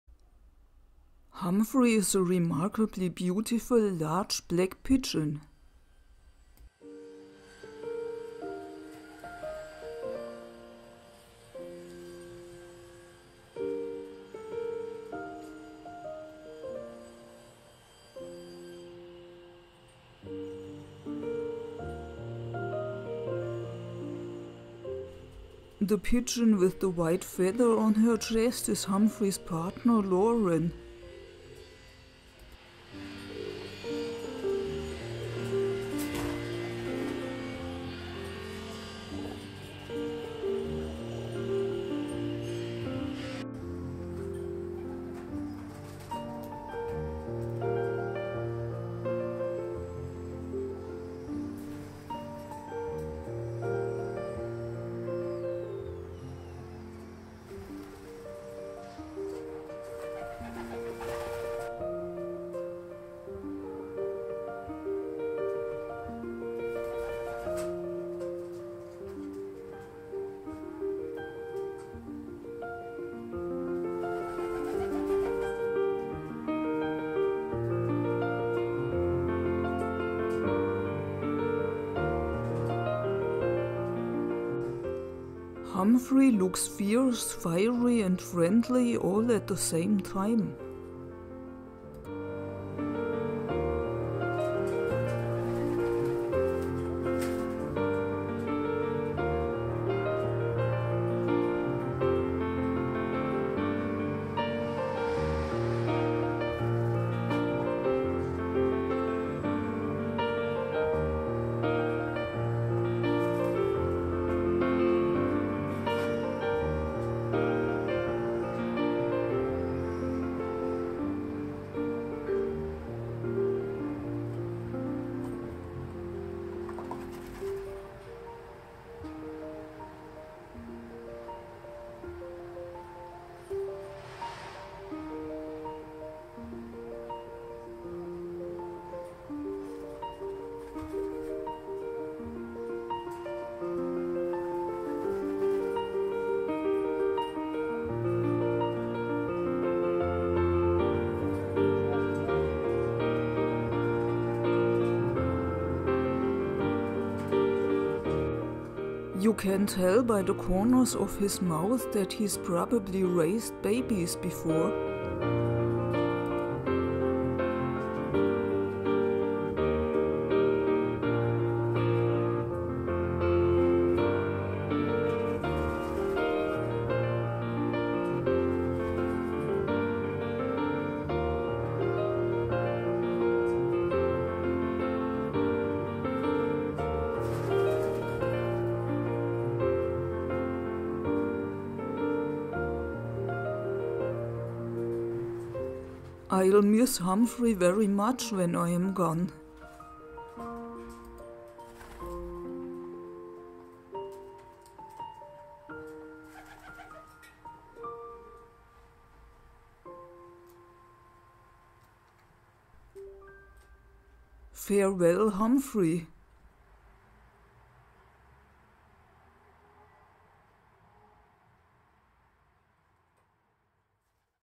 Humphrey, A Remarkable Pigeon
This is Humphrey, a particularly beautiful pigeon.
I'll miss him when I'm gone 😢
There are also cute animal videos: @ViktoriaGebser and @VictoriaGebser